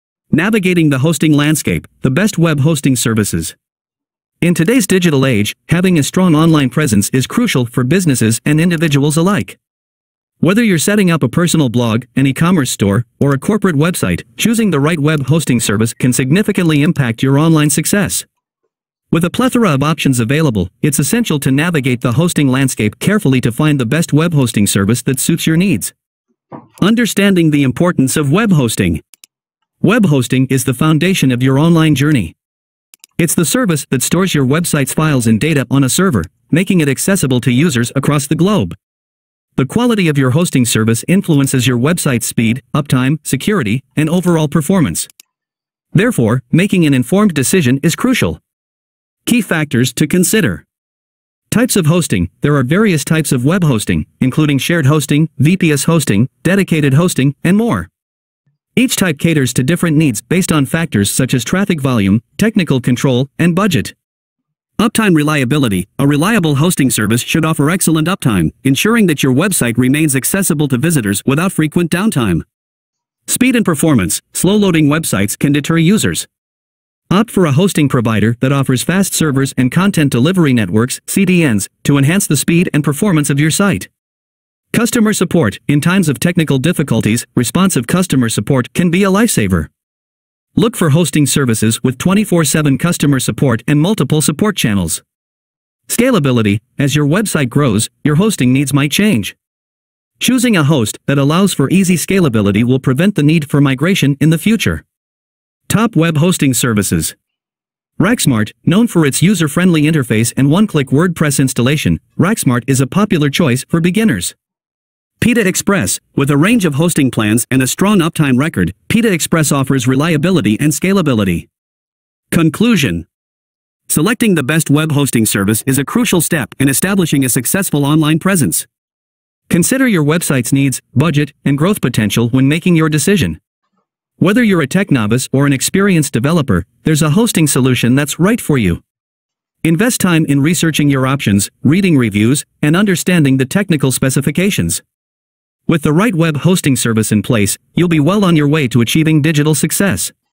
Best Web Hosting Services - Raksmart
In this video, we delve into the world of web hosting services and explore how to choose the best one for your website. Discover the key factors to consider, such as types of hosting, uptime reliability, speed, customer support, and scalability. We also highlight some top web hosting services like Raksmart and PetaExpress. Whether you're a beginner or an experienced user, finding the perfect hosting fit is essential for your online journey.